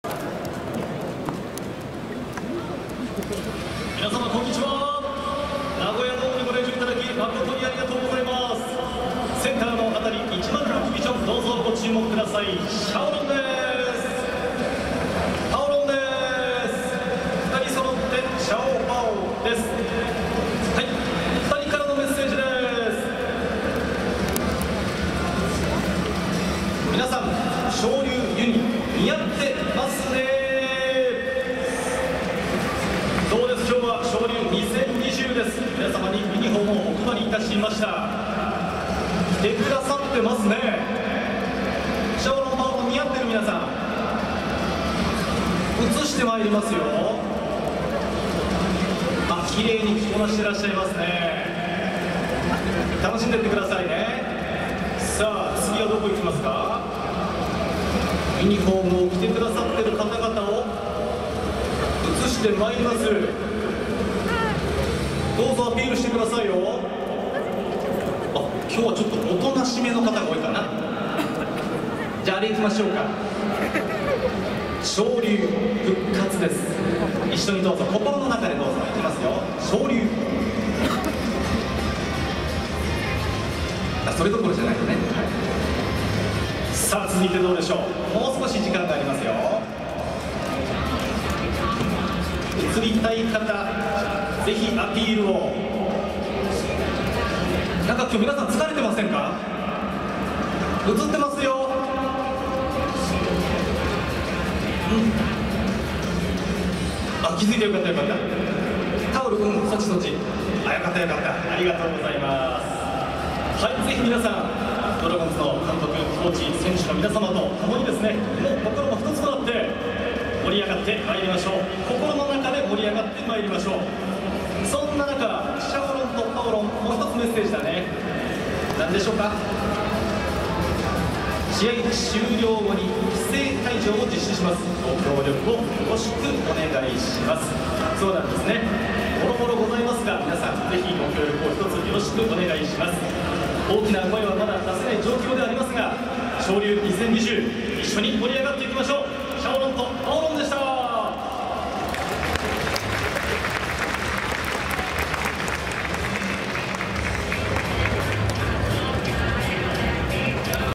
20/09/22 昇竜2020：試合開始前にシャオロンとパオロンからお願い
OPダンス前にシャオロンとパオロンからお願いがありました